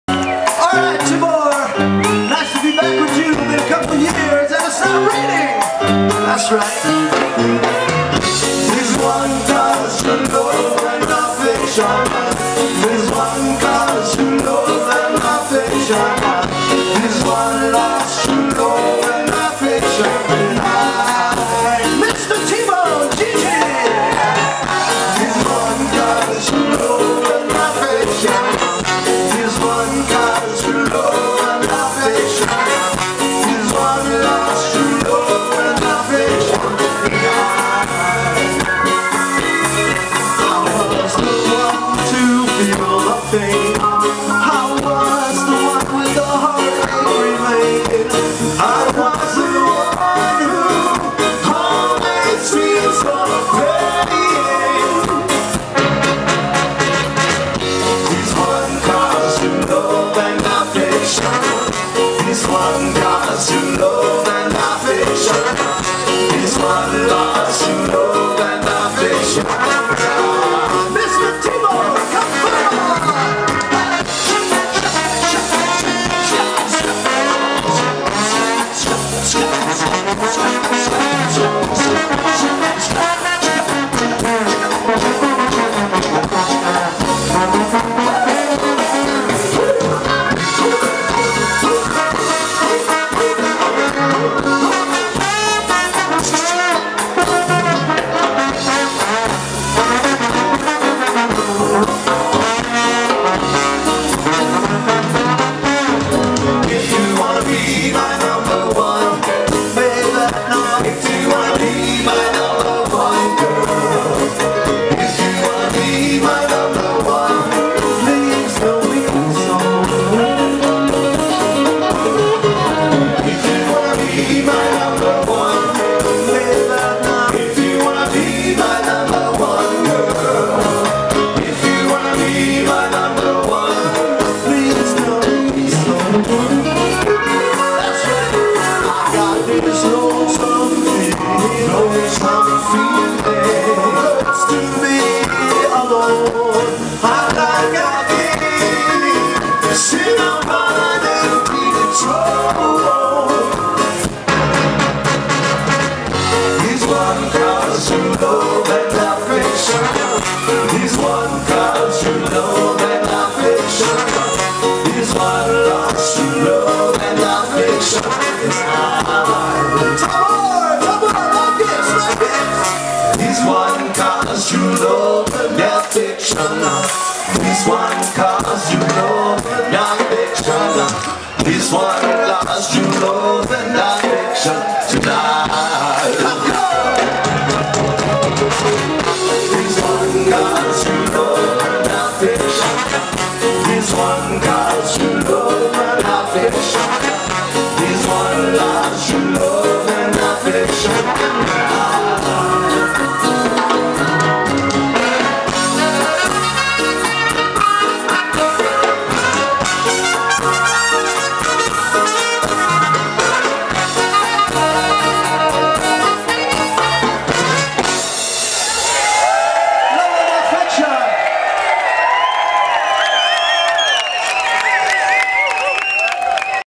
New York Ska Jazz Ensemble - Live at Mighty Sounds 2013
Záznam z 9. ročníku festivalu Mighty Sounds, který proběhl 19. - 21. července v Táboře.